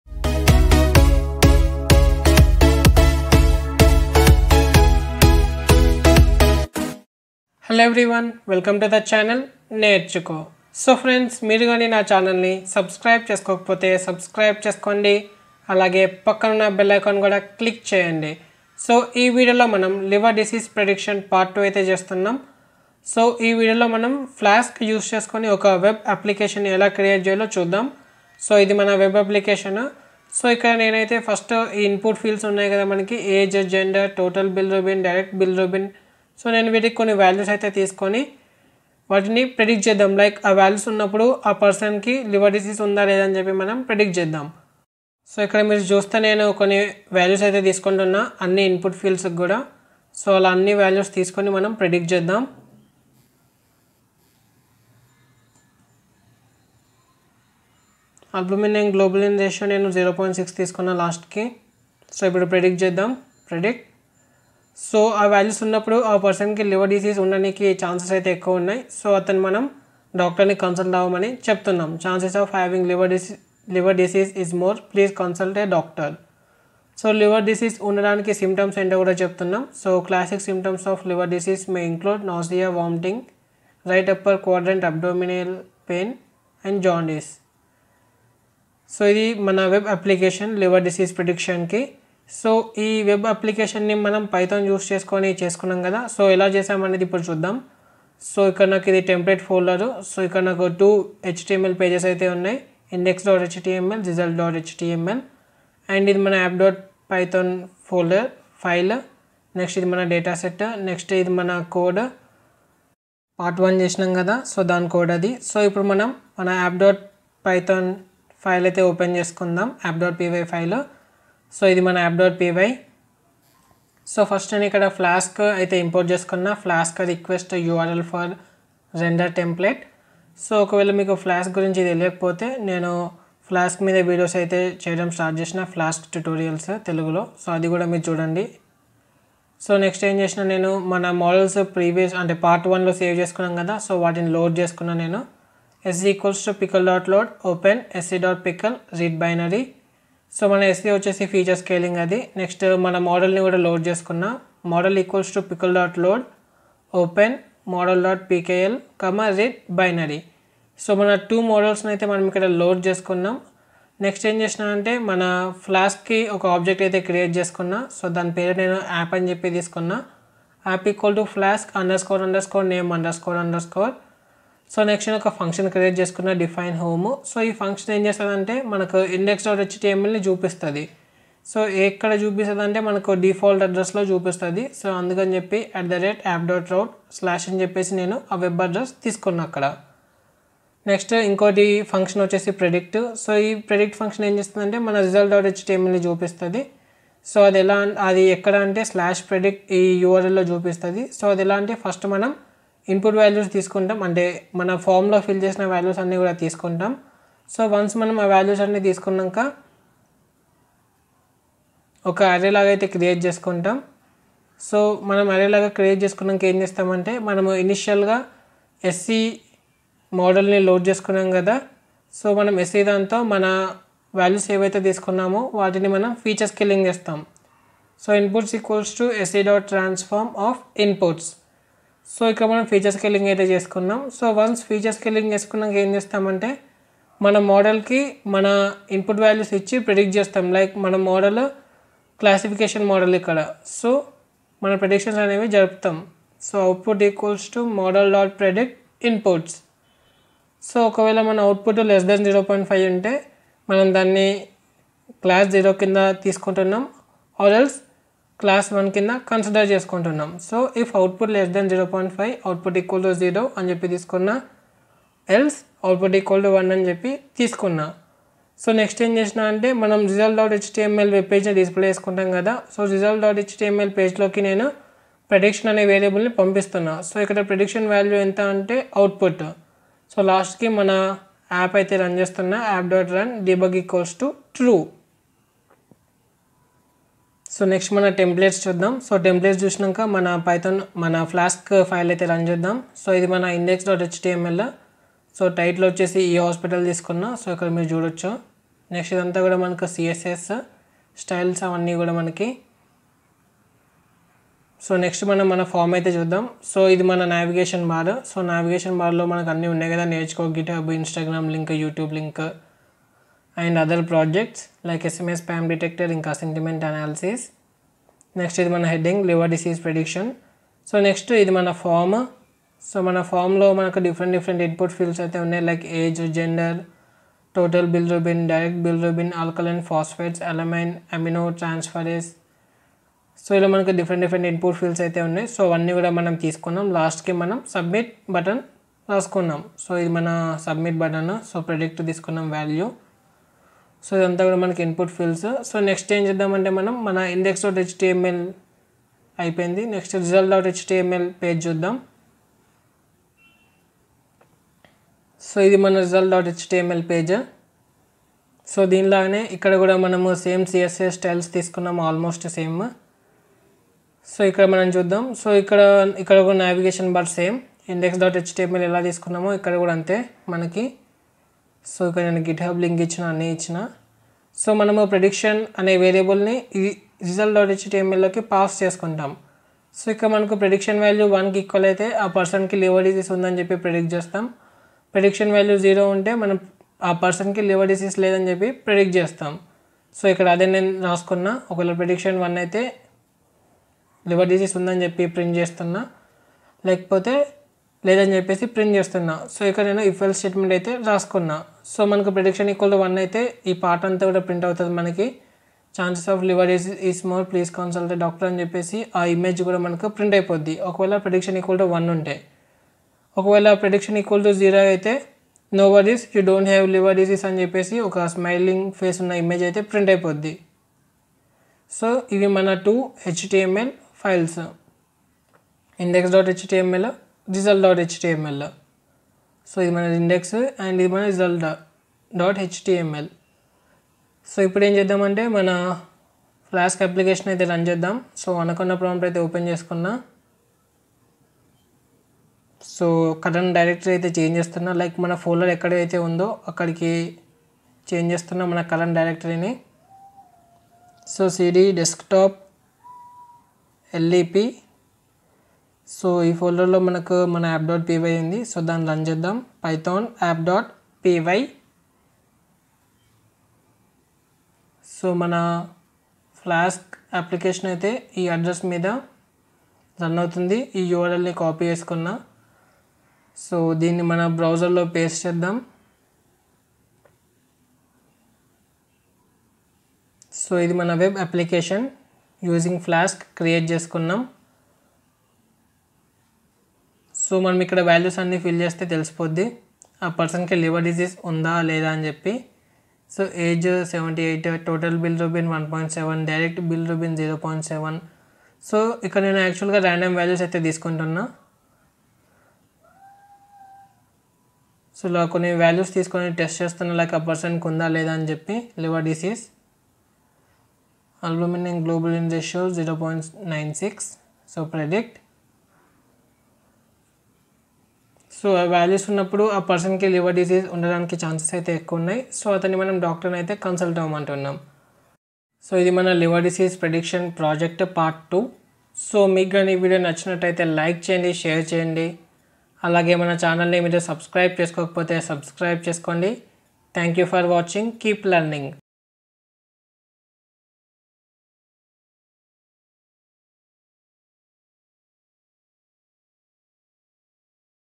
Liver Disease Prediction (Part - 2) in Telugu || Machine Learning in Telugu || Flask || Nerchuko
Hey folks,

In this video, we are doing a Liver Disease prediction project using machine learning techniques in Telugu